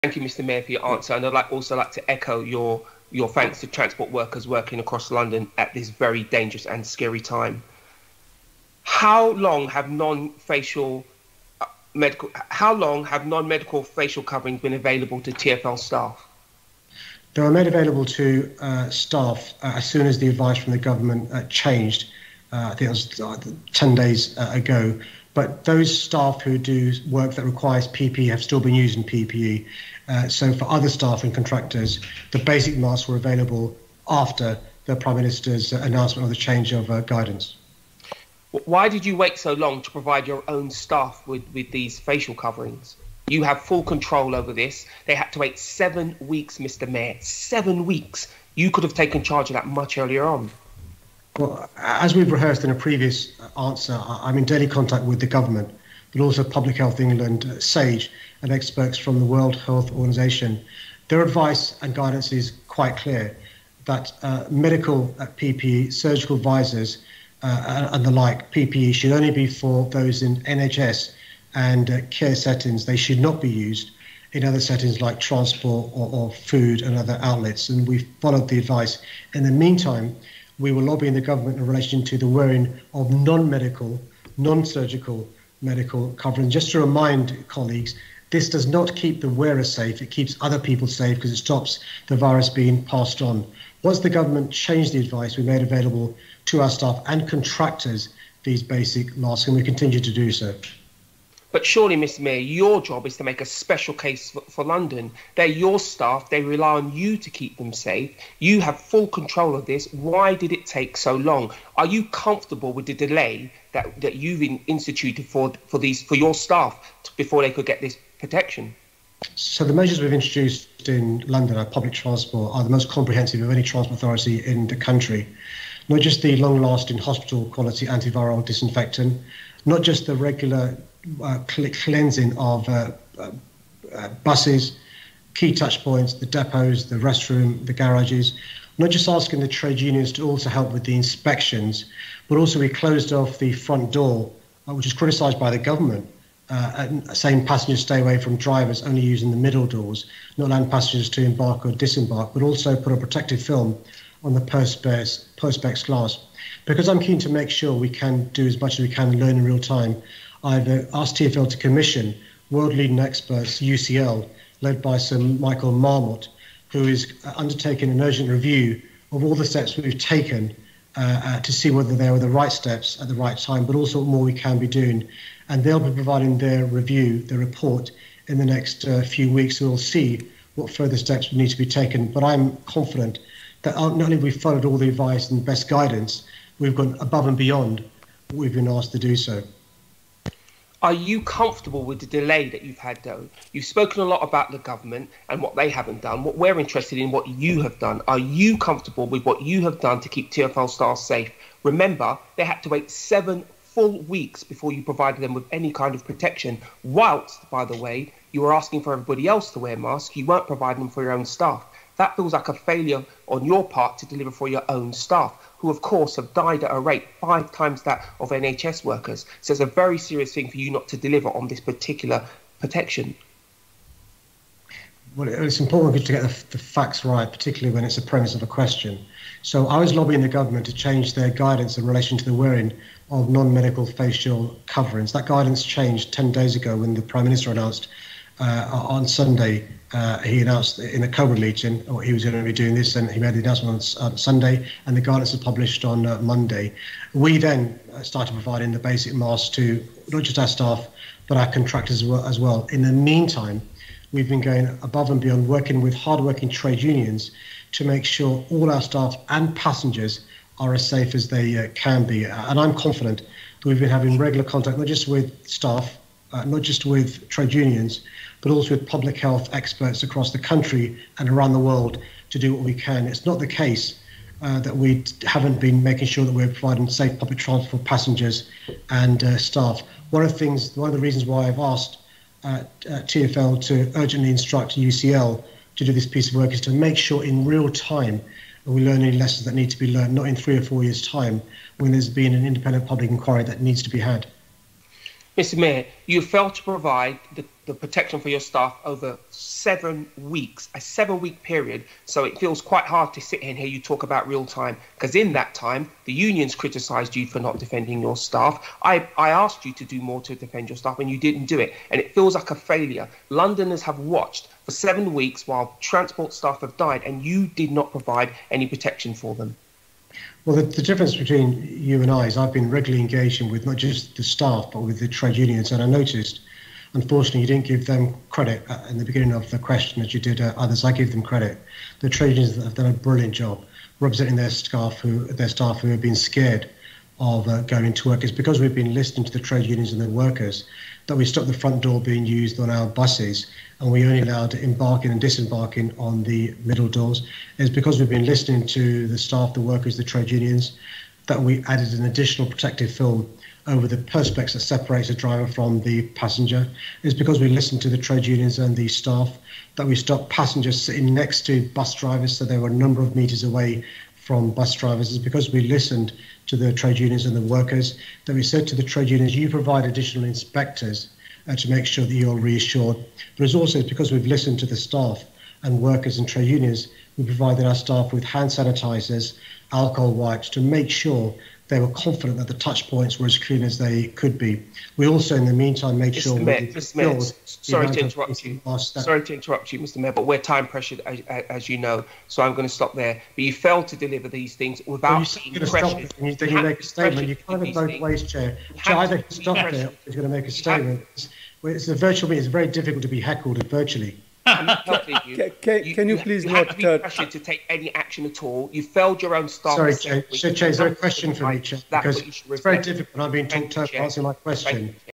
Sadiq Khan has failed to keep TfL staff safe during coronavirus outbreak | Mayor's Question Time
Shaun Bailey AM asks the Mayor why it took SEVEN WEEKS for him to provide Transport for London staff with face masks to protect their colleagues and commuters during the coronavirus outbreak.

Mayor's Question Time | 21st May 2020